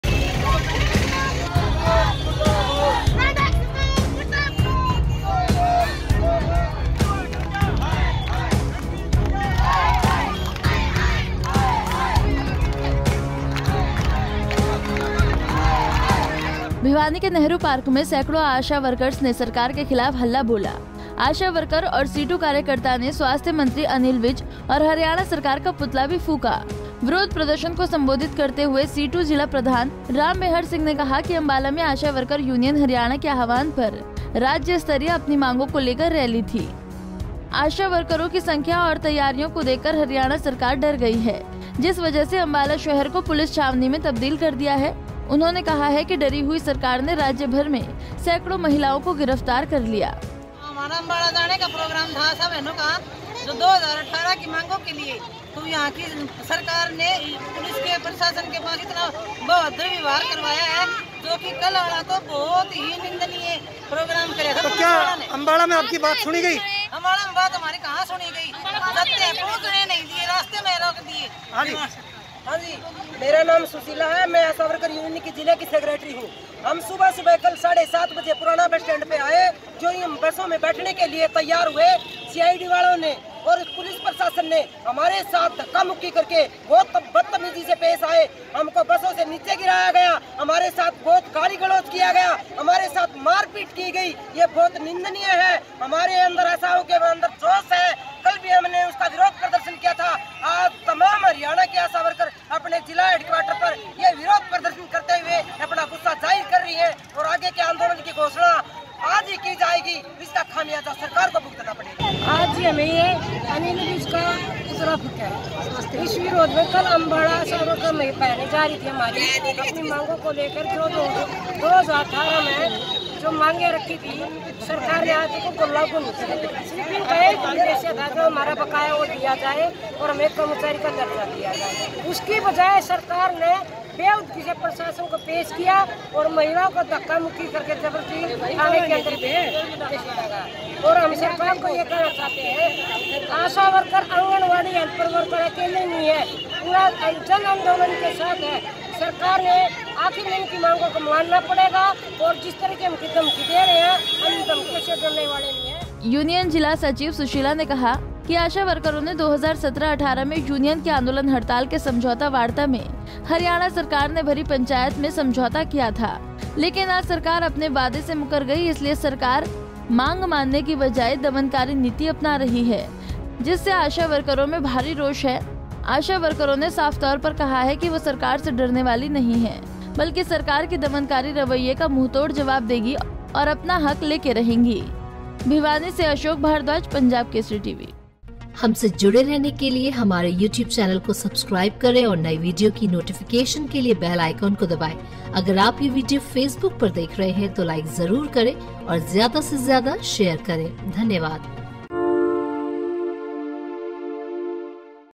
Bhiwani में Asha Workers ने किया जोरदार प्रदर्शन, Anil Vij और राज्य सरकार का फूंका पुतला
भिवानी के नेहरू पार्क में सैकड़ों आशा वर्कर्स ने सरकार के खिलाफ हल्ला बोला. आशा वर्करों और सीटू कार्यकर्ताओं ने स्वास्थ्य मंत्री अनिल विज और हरियाणा सरकार का पुतला भी  फूंका...विरोध प्रदर्शन को सम्बोधित करते हुए सीटू जिला प्रधान राममेहर सिंह ने कहा कि अंबाला में आशा वर्कर यूनियन हरियाणा के आह्वान पर राज्य स्तरीय अपनी मांगों को लेकर रैली थी.